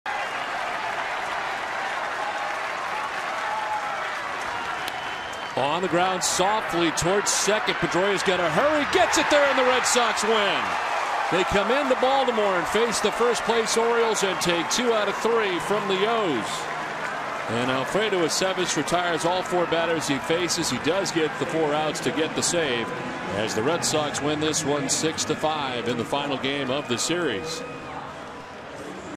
2012/05/23 Aceves notches the save
BOS@BAL: Aceves gets Avery to groundout to end it